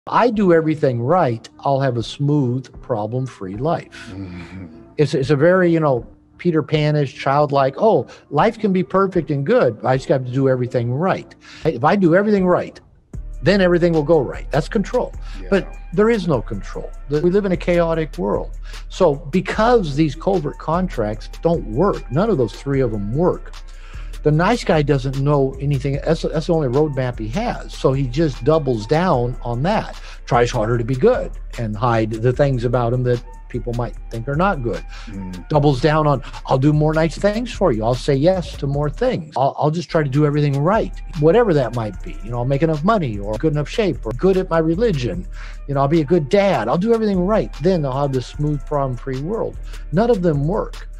NICE GUYS & Covert Contracts w Dr. Robert Glover
Episode 64 - No More Mr. Nice Guy w/ Dr. Robert Glover